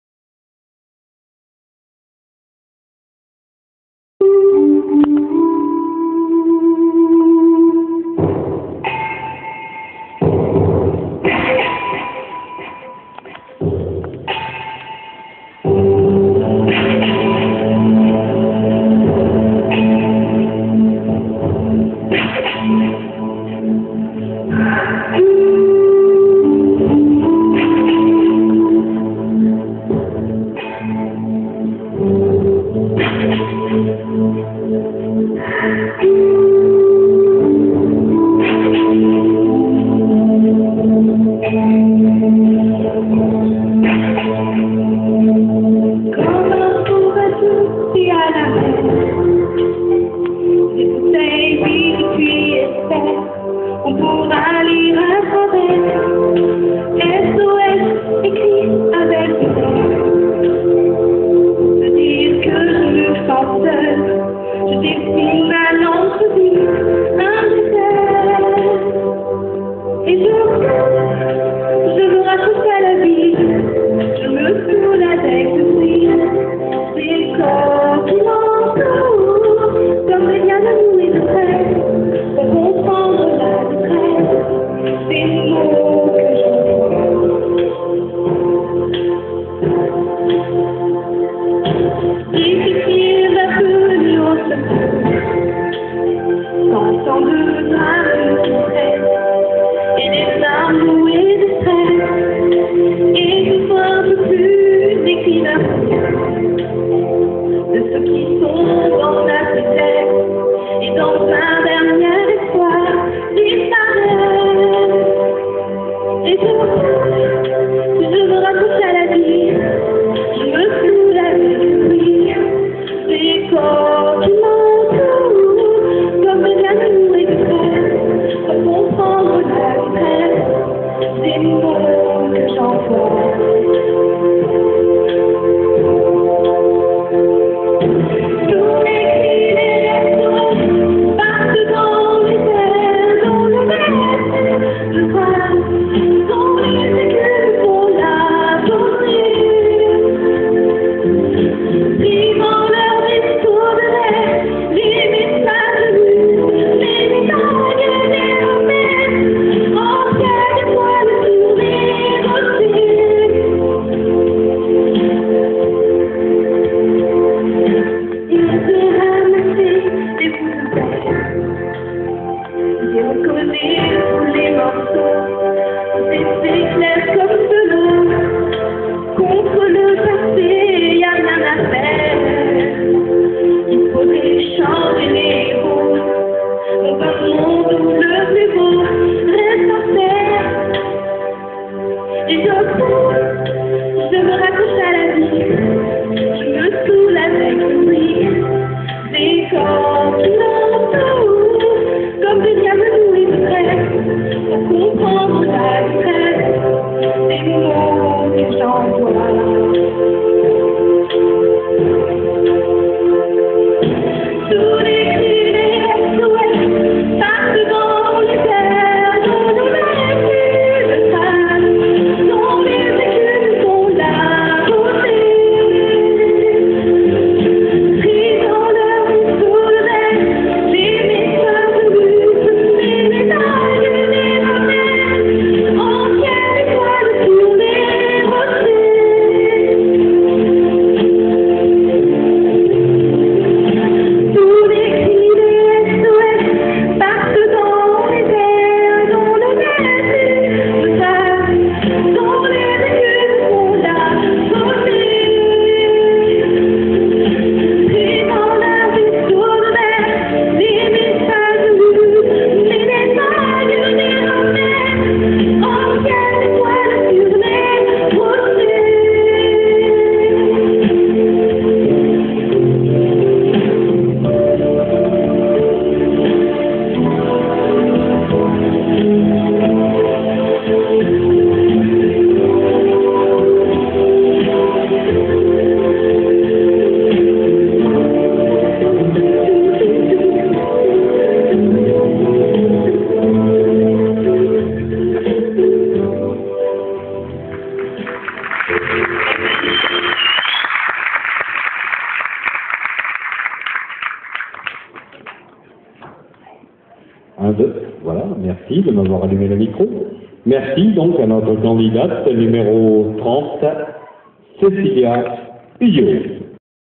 la biche dor 2007 categorie adolescent
un dossier sur moi c une catastrophe
vive des micro mal regler